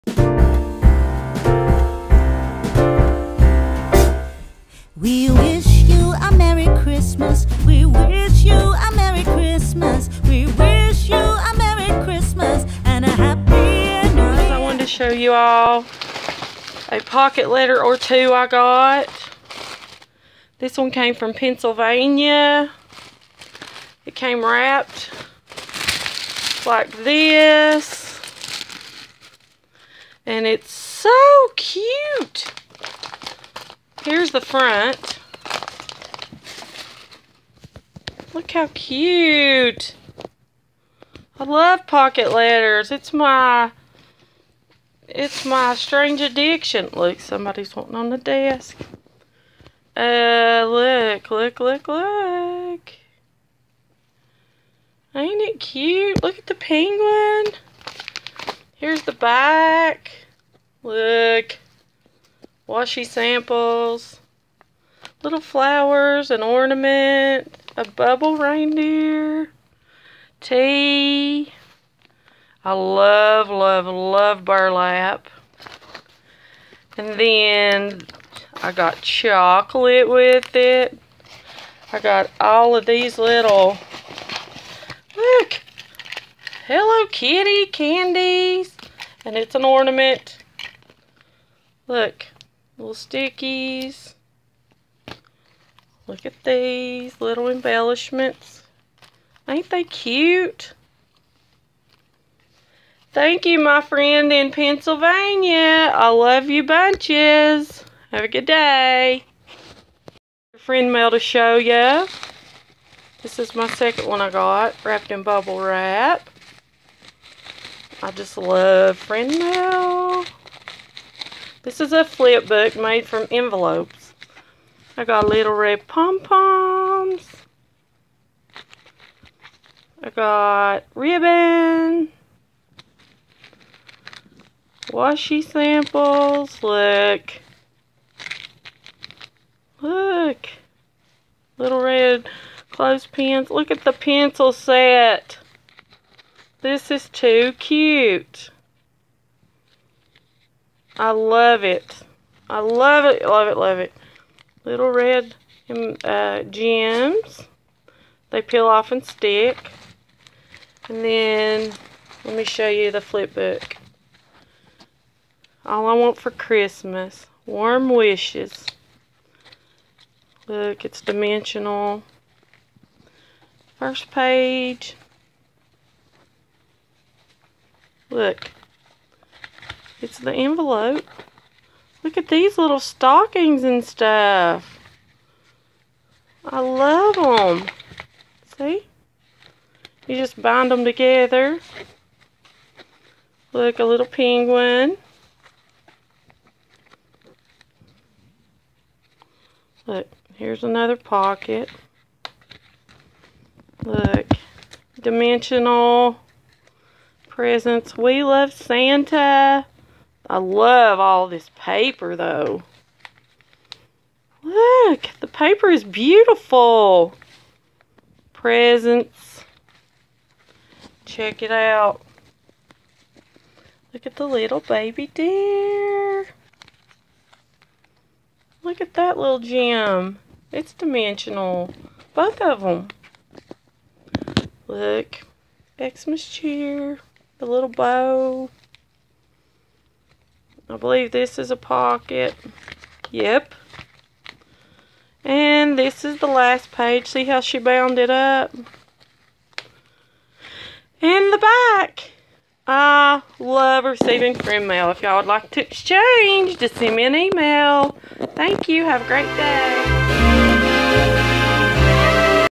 ~~FRIENDMAIL | Thanks so much!!
A couple friend mails I received over the weekend. I love to pocket letters and flip book/bag swaps!

Much Love, TamTam
x0x0x